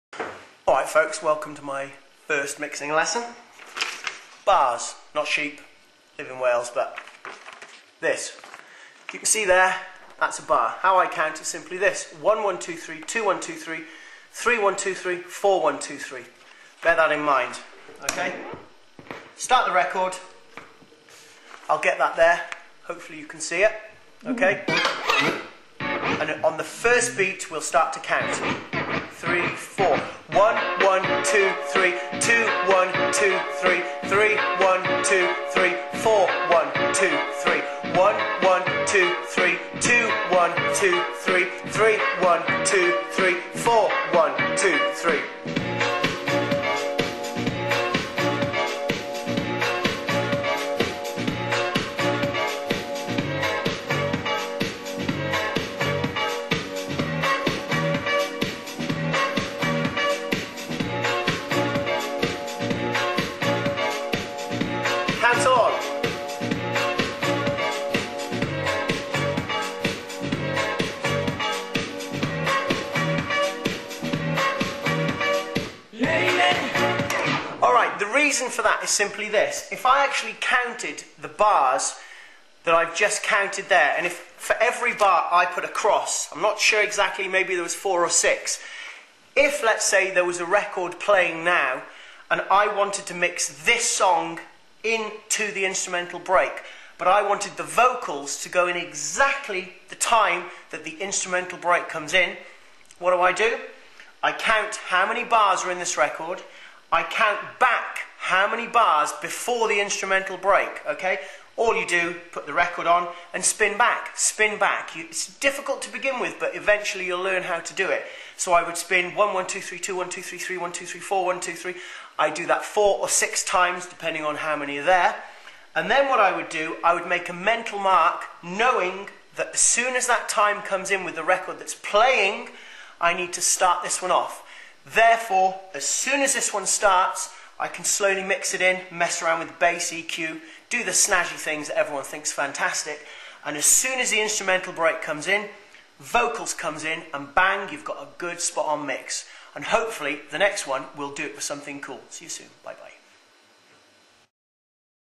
the original video, mistake on the bar it's 1,2,3,4 not as said in video!
PLEASE READ! a bar is counted 1,2,3,4, but using this method greatly reduces the counting and so helps to better understand the structure of the record/CD.
 thanks